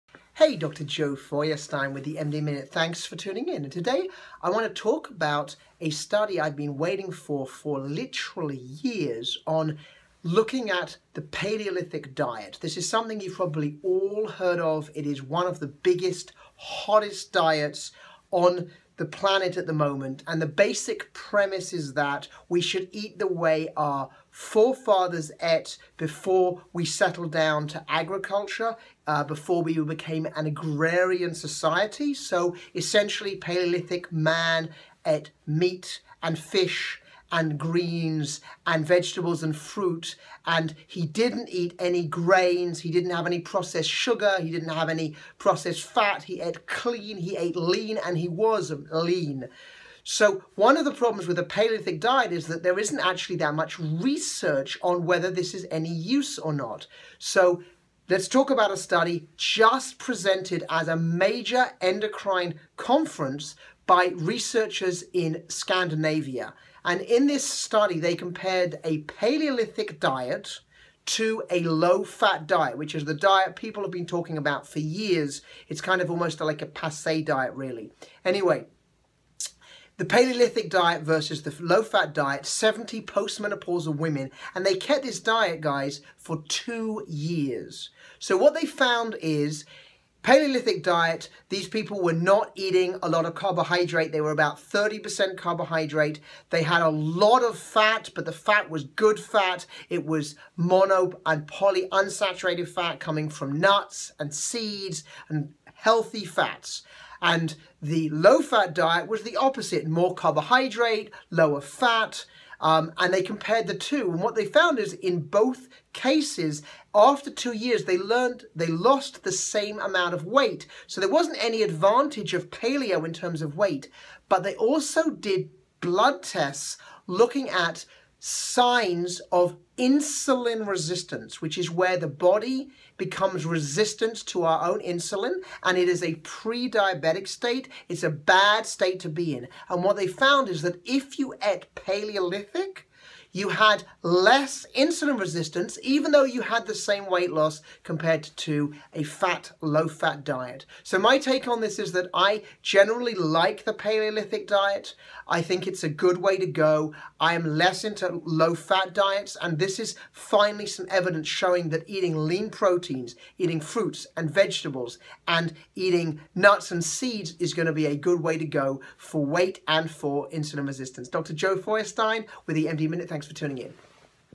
Paleo Diet - Finally a clinical study on the health benefits
Paleo Diet - Finally a clinical study on the health benefits - Dr. Joe Feuerstein

Paleo Diet - Finally a clinical study on the health benefits - Dr. Joe Feuerstein. Unlock the secrets of the Paleo Diet with groundbreaking clinical research! Discover the remarkable health benefits unveiled in this study. Explore the science behind this ancient approach to nutrition.

In this video Dr. Joe Feuerstein the Paleo Diet, often referred to as the "caveman diet," emphasizes the consumption of whole foods like lean meats, fish, fruits, vegetables, nuts, and seeds while avoiding processed foods, grains, and dairy.

A clinical study on the health benefits. Proponents argue that by returning to our evolutionary roots, we can potentially reduce the risk of chronic diseases and experience improved vitality. However, until recently, its effectiveness remained largely anecdotal.

The latest cutting-edge research on
Dr. Joe Feuerstein
The Medical Minute